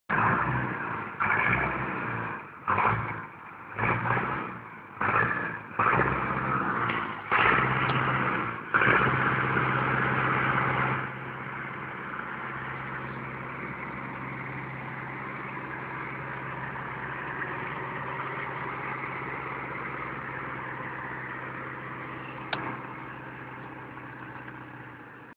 Pajero pulling tractor and trailer uphill on dirt
Tractor's clutch failed. Need to tow it to the workshop.